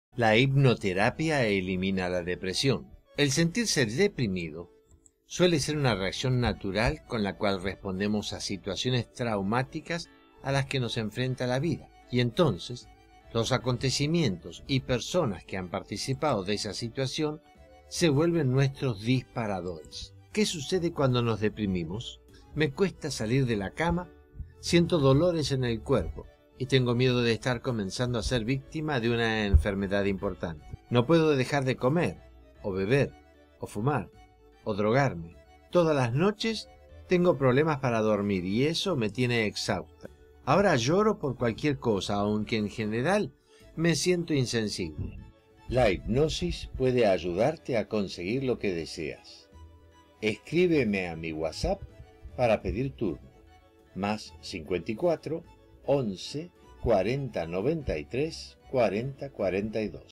La Hipnoterapia Elimina La Depresion
Inscríbete aquí para recibir los videos gratis del Dr. Bonomi

Sentirse deprimido es una reacción natural frente a situaciones traumáticas
Cuando nos deprimimos, nos cuesta salir de la cama, sentimos dolores en el cuerpo, no podemos dejar de comer o beber o fumar o tomar drogas, tenemos problemas para dormir y hasta lloramos por cualquier cosa